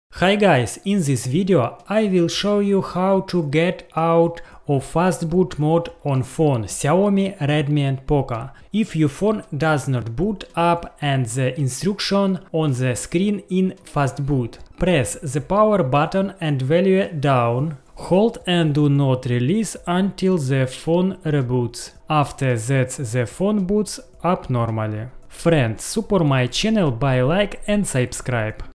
XIAOMI HOW TO EXIT FASTBOOT
In this video I will show you how to get out of fastboot on phones: Xiaomi, Redmi, Poco.
If your phone does not boot up and the inscription on the screen is fastboot
Press the power buttons and volume down,
  hold until the phone reboots.
After that, the phone boots up normally.